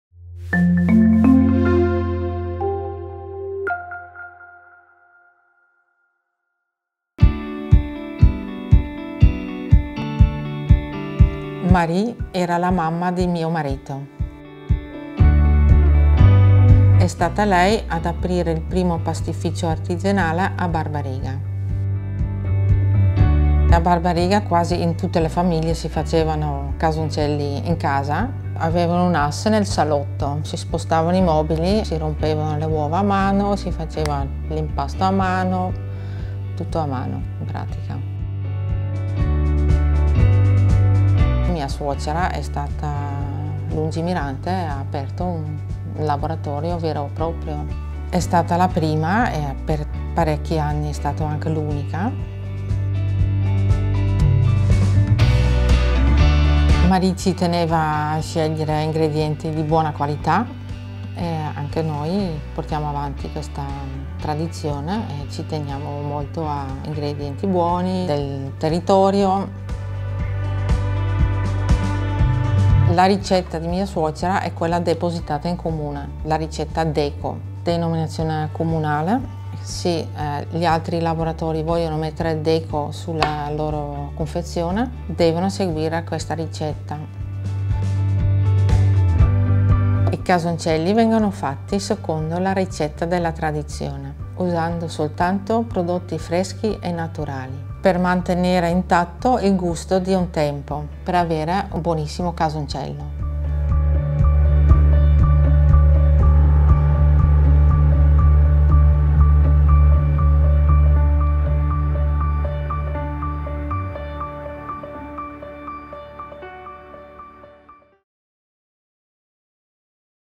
Gardaamore.it - Local Ambassador: Casoncelli Marì
Vi presentiamo un nostro Local Ambassador, Corinne, grazie alla ricetta che fù della suocera Marì, rappresenta la tradizione della pasta fresca fatta a mano a Barbariga (BS).

In questo piccolo borgo bresciano nasce il miglior casoncello di tutta la Provincia di Brescia, frutto di una selezione sapiente delle materie prime e forti di una lavorazione completamente artigianale e che cura nei dettagli la creazione del ripieno e della pasta stessa.

Il video che vi proponiamo non è un video realizzato per fini pubblicitari, ma rappresenta in maniera veritiera i processi di produzione fatti nel piccolo laboratorio di Barbariga.

Una piccola produzione, ma un'altissima qualità che da valore al territorio del Lago di Garda e del suo entroterra... da giugno sarà possibile anche scoprire dal vivo i segreti del pastificio Casoncelli Marì con le "Local Experience di Gardaamore"

Grazie a Corinne che guida questa attività e fa parte di Gardaamore.

Gardaamore ti accompagna in un viaggio alla riscoperta delle eccelenza nel Lago di Garda e nel suo stupendo entroterra.

Gardaamore...segui il cuore, make the difference!